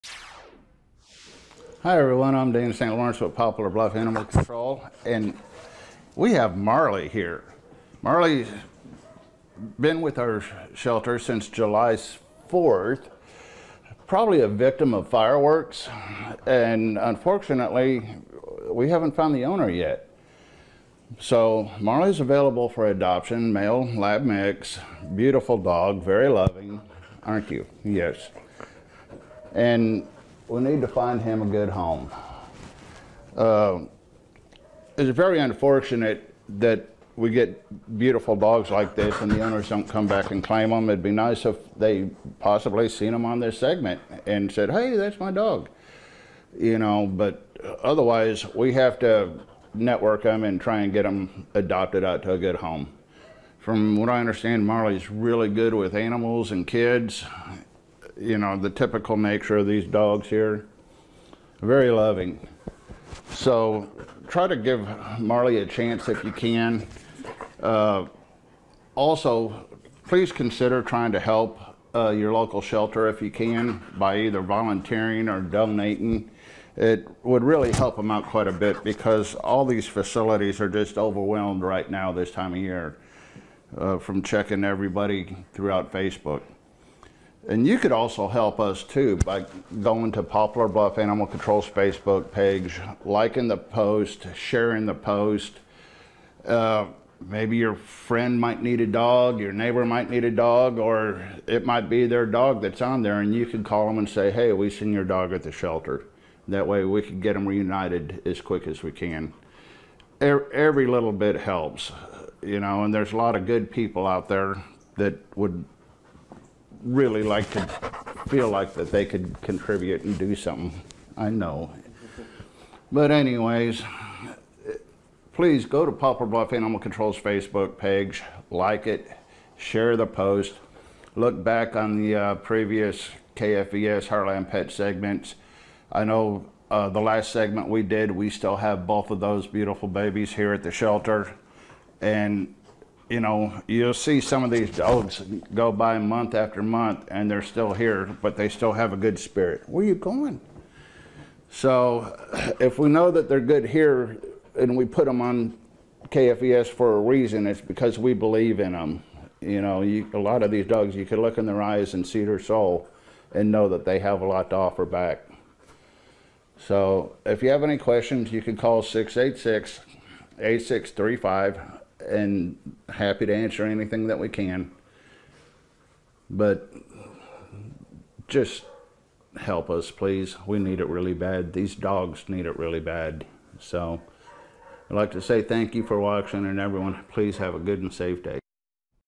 Heartland Pets feat. Marley 7/28
You can adopt Marley from the Poplar Bluff Animal Control.